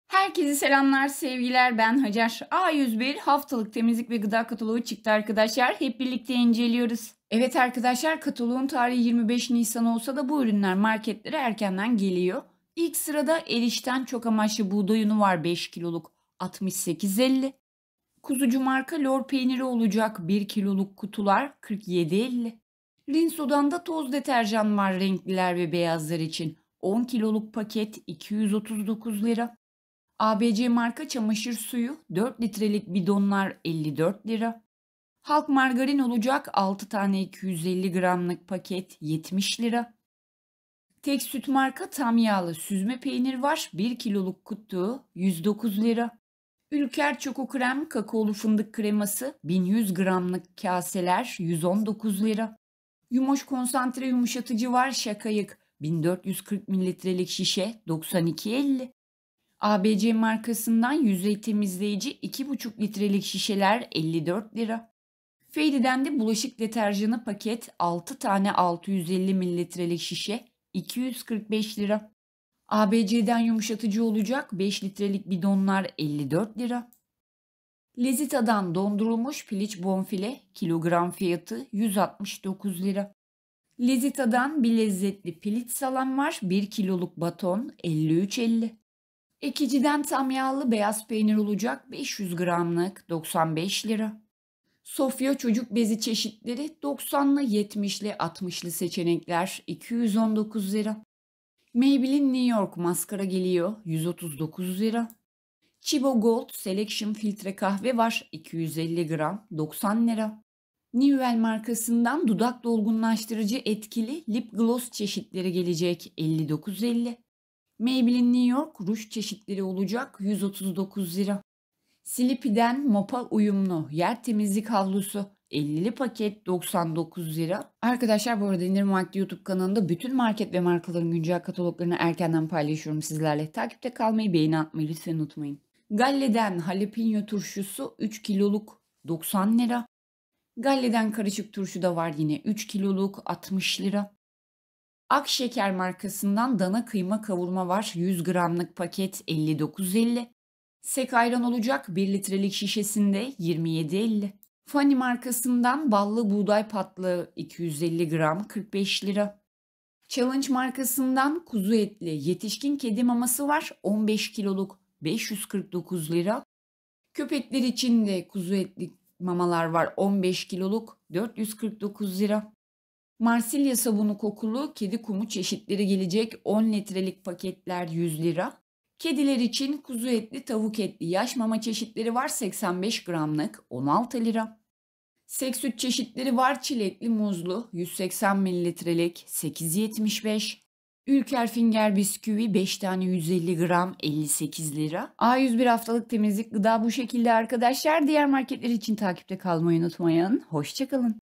A101 HAFTALIK FIRSAT TEMİZLİK GIDA KATALOĞU | A101 25 NİSAN 2024 | A101 BU HAFTA İNDİRİM VE KAMPANYA
A101 bu hafta temizlik gıda ürünlerini tekli sunumla inceliyoruz! A101 25 Nisan 2024 kataloğu içerisinde yer alan tüm ürünler kampanyalar İndirim vakti youtube kanalında!

ABC Çamaşır Yumuşatıcı
Yumoş Konsantre Yumuşatıcı
Rinso Toz Deterjan
Fairy Sıvı Bulaşık dEterjan 6'lı
Ekici Tam Yağlı Peynir
Eriş Un 5 kg
Halk Margarin paket 6'lı
Maybelline Newyork Maskara
Maybelline Newyork Ruj Çeşitleri
dudak Dolgunlaştırıcı Lip Gloss
Çokokrem 1100 g
Finger bisküvi
Challenge Kedi & Köpek Maması 15 kg
Tchibo Filtre Kahve
Akşeker Dana kıyma Kavurma